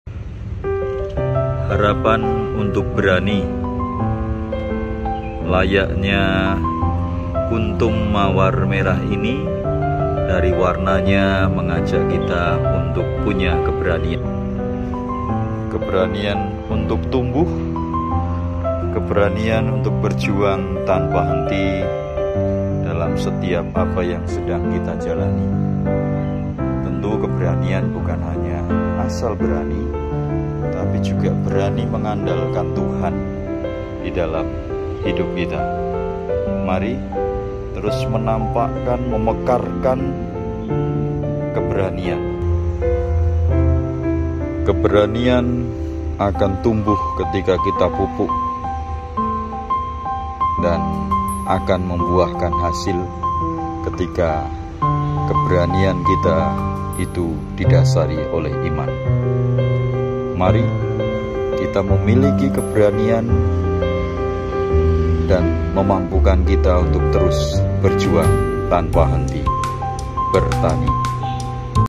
courtesy of

Sharing Aktifitas di St Yusup : inspirasi berani dari kuncup mawar merah
menjadi berani adalah bagian dari semangat BerTani : Berjuang Tanpa Henti.
Selamat beraktifitas, Tuhan memberkati dan memberi kita damai.